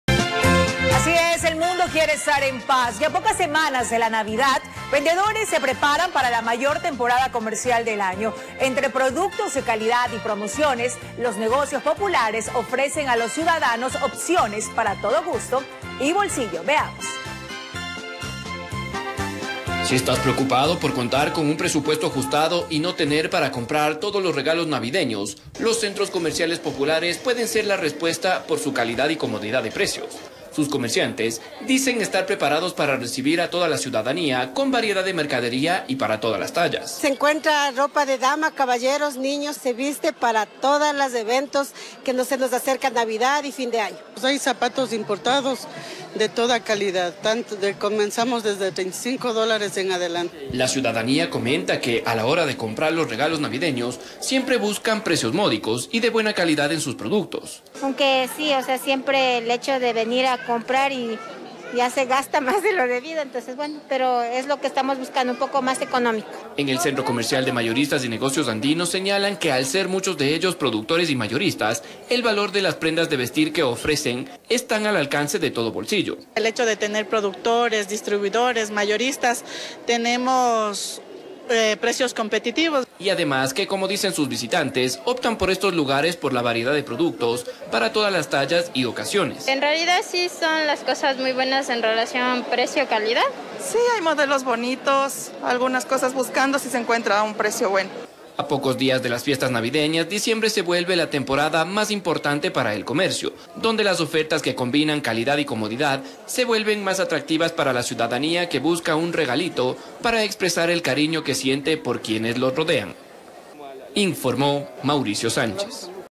Aumenta comercio previo a navidad
A pocas semanas de la navidad, los vendedores se preparan para la mayor temporada comercial del año. Entre productos de buena calidad y grandes promociones, los centros comerciales populares brindan una gran variedad de artículos e intereses. En el Centro Comercial de Mayoristas y Negocios Andinos ofrecen precios al alcance de todos al ser un centro de productores y mayoristas.